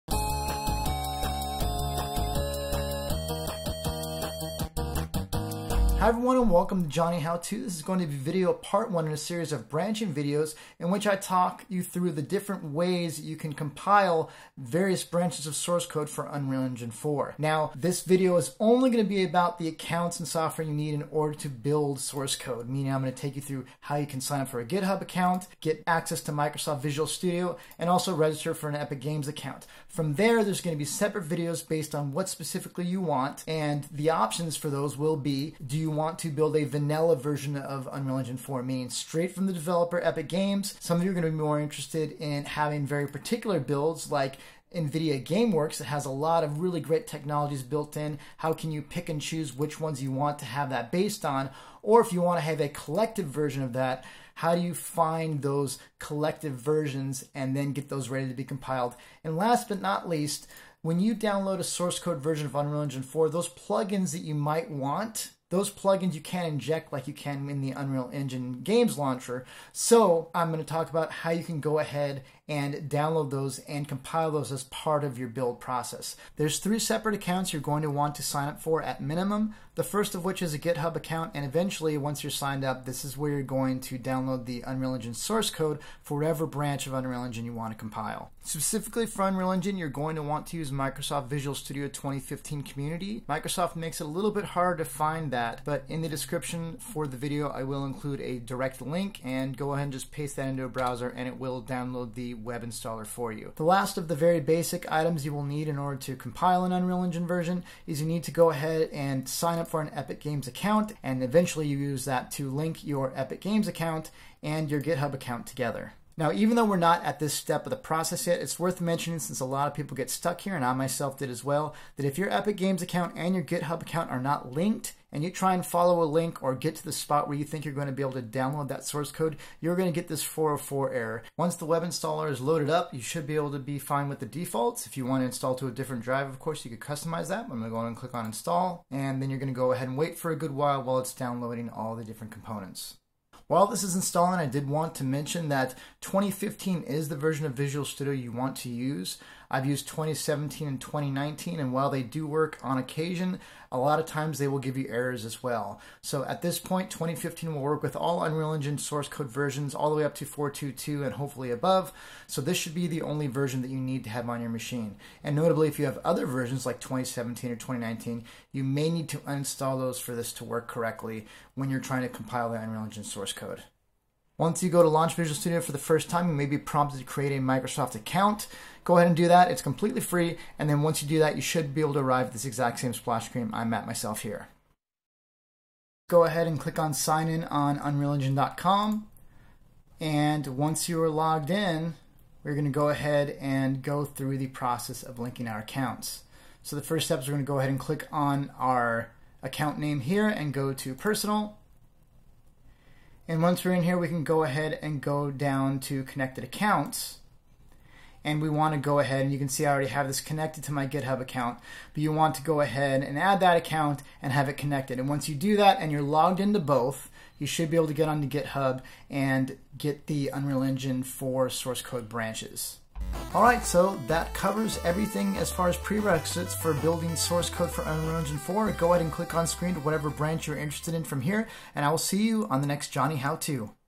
Compile Unreal Engine from Source Code - Part 1: Prerequisites
Links below this description!
This video is part of a "branching" series covering the process of signing up, finding, and building Unreal Engine 4 from original source code with different technologies built-in.

This particular video focuses on the required accounts and (free) software you will need to build the source code for any Unreal Engine 4 build.

Branching "Part 2's" (choose which you want!)...